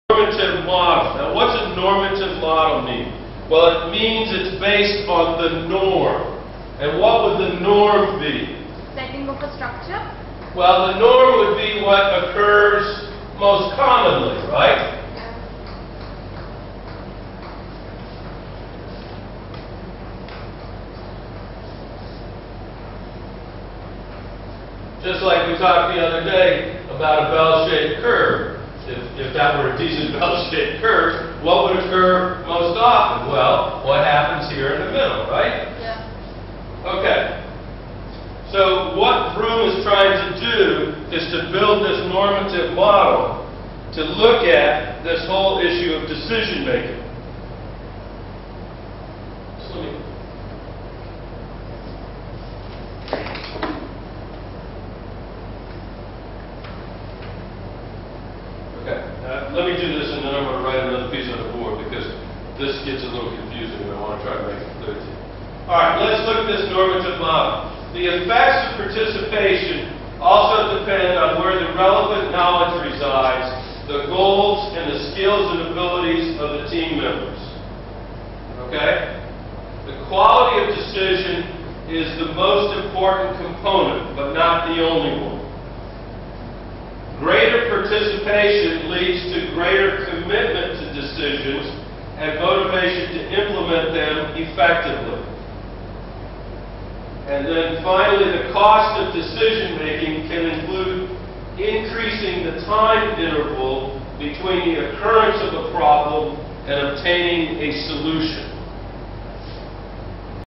IILM, Foreign Faculty Lecture by (Prof. Scott R.Lyman, Winthrop University)
IILM, Foreign Faculty Lecture by (Prof. Scott R.Lyman, Winthrop
University, USA. Lecture On (Leadership Development)